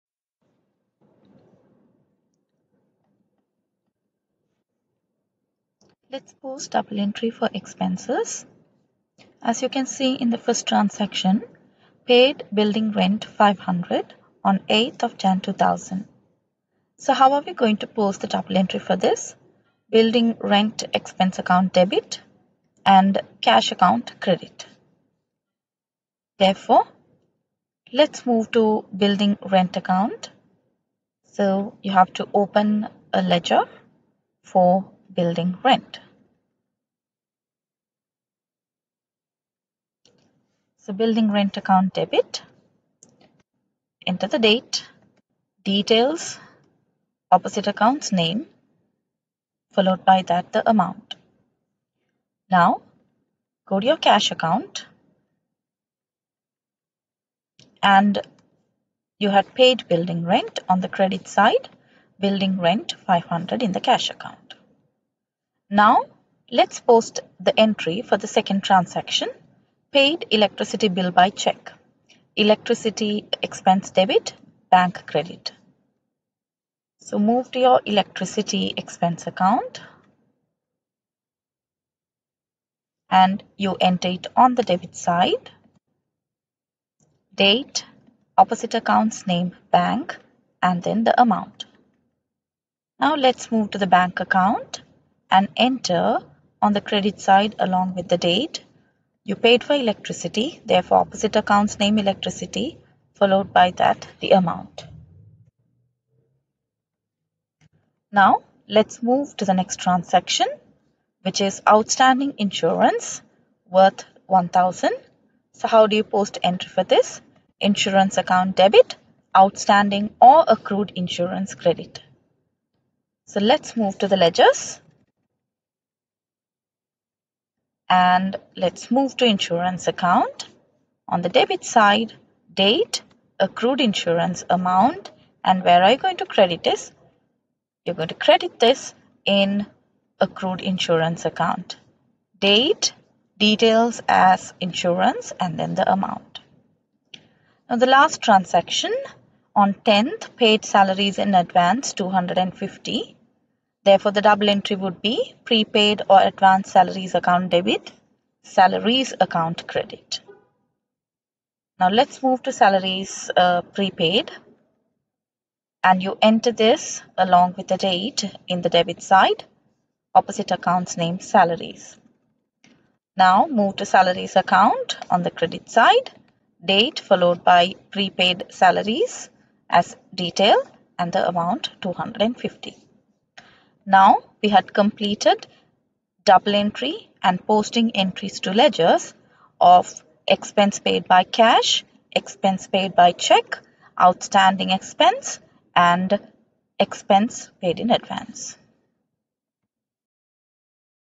Double entry for expenses
- Double entries:
1. Expenses made by cash
2. Expenses made by cheque
3. Expenses outstanding
4. Expenses prepaid
- Posting entries to ledgers